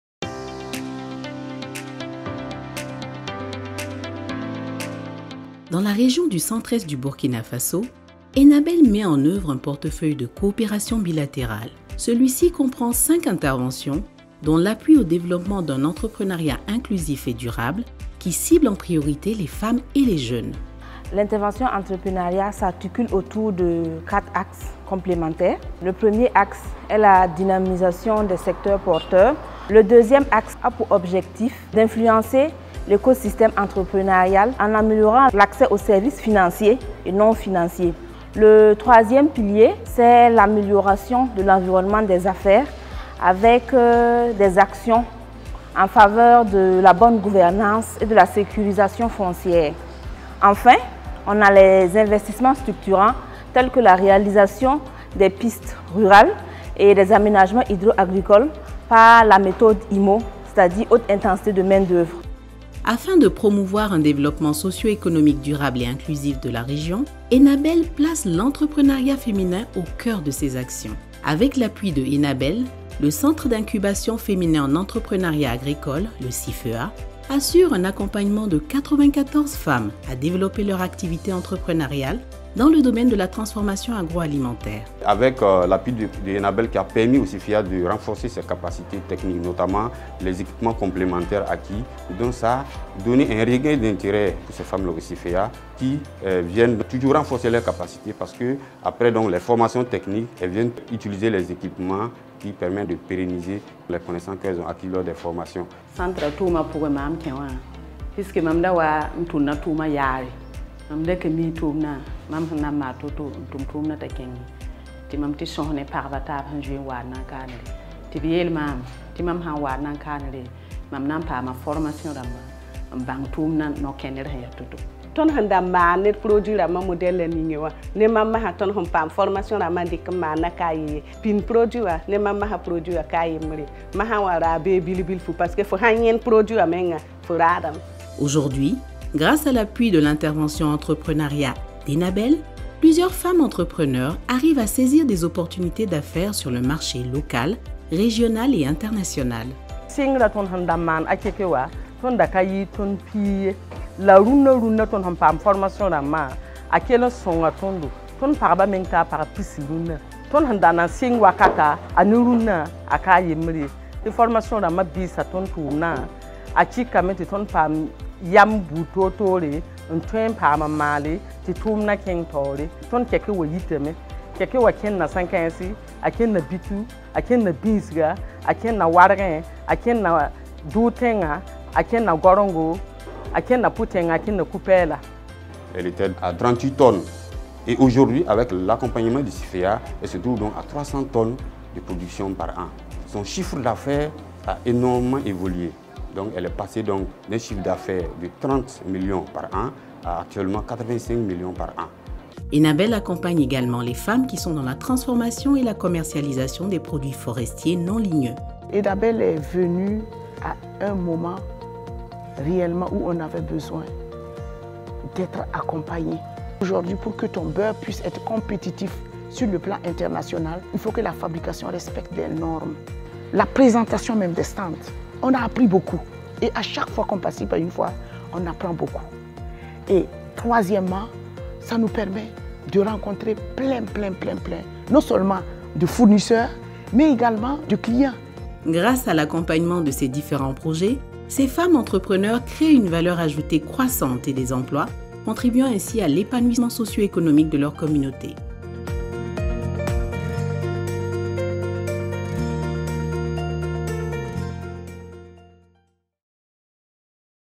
Enabel en het vrouwelijke ondernemerschap in Burkina Faso
De werkgelegenheid in Burkina Faso is onzeker, met een groeiende werkloosheid en een uittocht van jongeren van het platteland naar de grote stedelijke centra op zoek naar werk.

Het grootste deel van de bevolking, vooral vrouwen, is actief in de agro-sylvo-pastorale sector en in de voedselproducerende landbouw.

In de regio Centre-East van Burkina Faso ontwikkelt Enabel een aanpak die met name vrouwen in staat stelt hun capaciteiten op het gebied van het scheppen van werkgelegenheid en economisch beheer te versterken. Dit verbetert het concurrentievermogen van hun micro-ondernemingen.

Ontdek met deze film de wereld van de strijdbare vrouwen in de regio Centre-East van Burkina Faso.